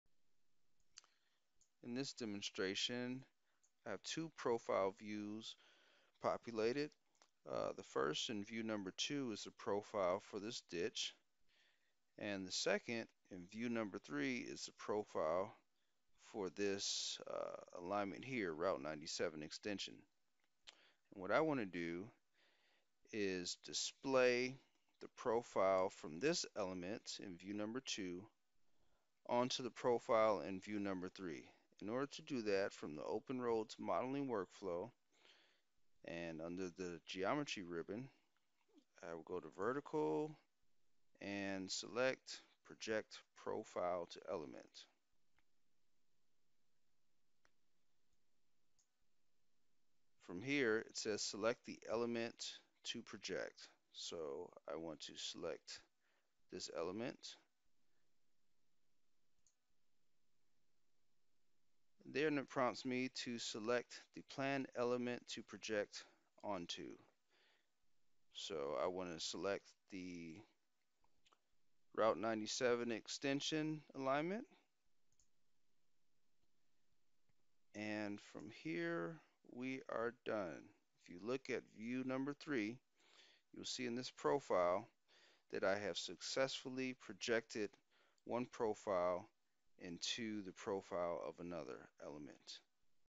ORD: Project Profile to Element
Video tutorial demonstrating the use of the "Project Profile to Element" tool in OpenRoads designer.